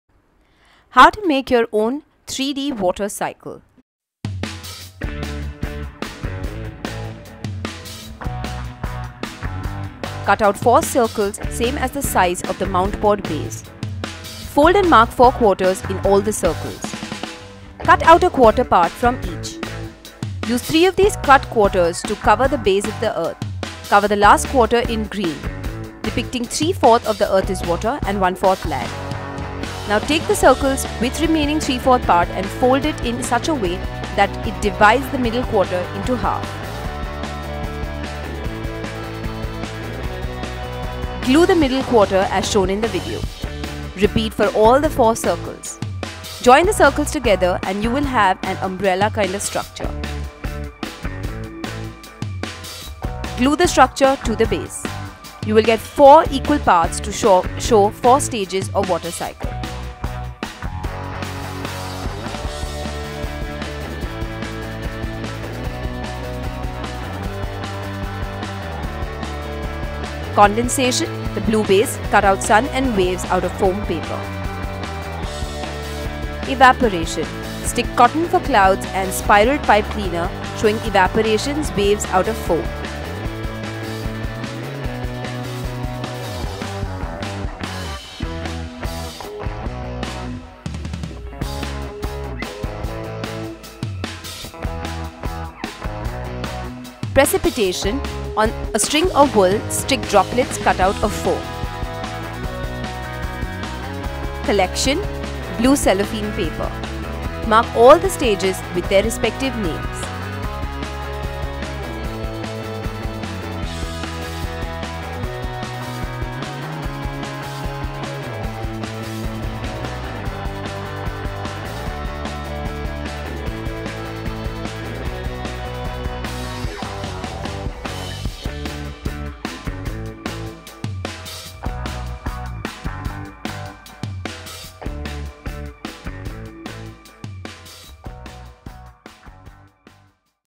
How To Make A 3D Water Cycle Model
Teaching your kids all about the water cycle? Then instead of answering questions show them what it is with this easy model of the water cycle. We tell you how to make it with  step by step instructions!